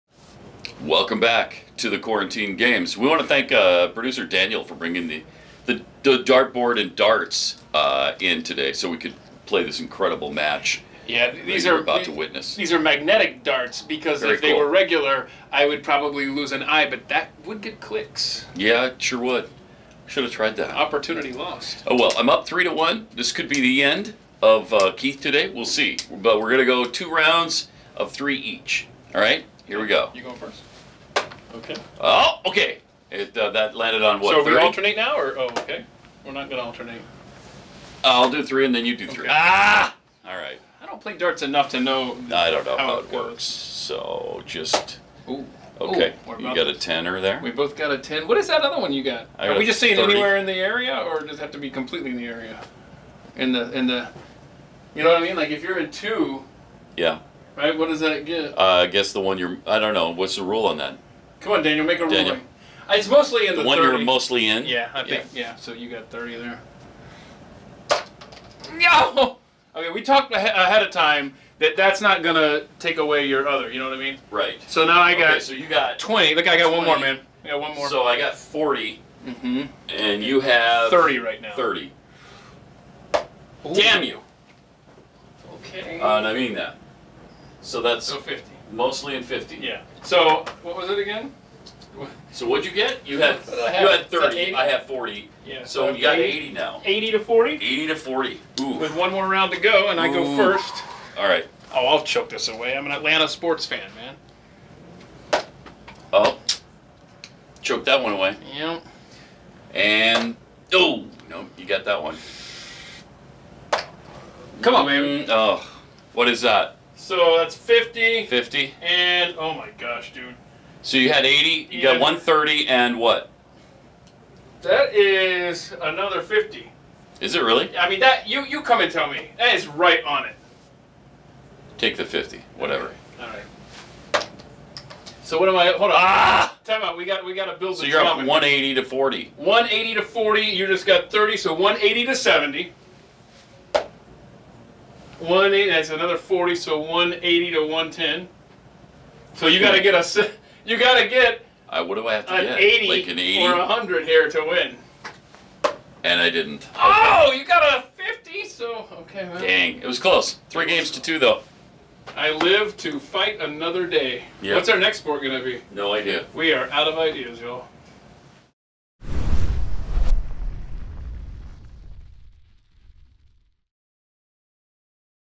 Quarantine Olympics Round 2 | Day 5 - Magnetic Darts
Pat and Keith are ready to go with Round 2 of our Quarantine Olympics!  This is Day 5 and they're playing Magnetic Darts!